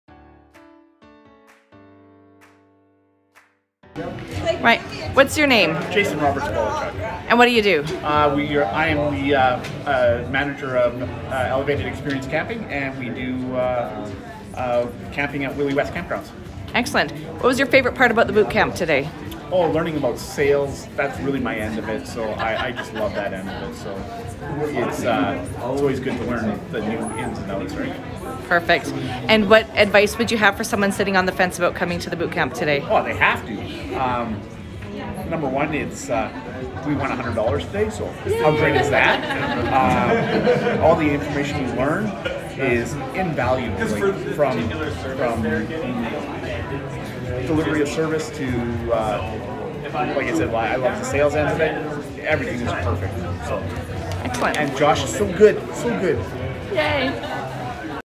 Business Bootcamp Reviews | Beat The Odds | Jason Roberts Kowalchuk February 29, 2020
In this video Jason Roberts Kowalchuk gives her honest review of the Spurrell & Associates CPA Beat The Odds Business Bootcamp. To learn more about the highest rated business bootcamp in Canada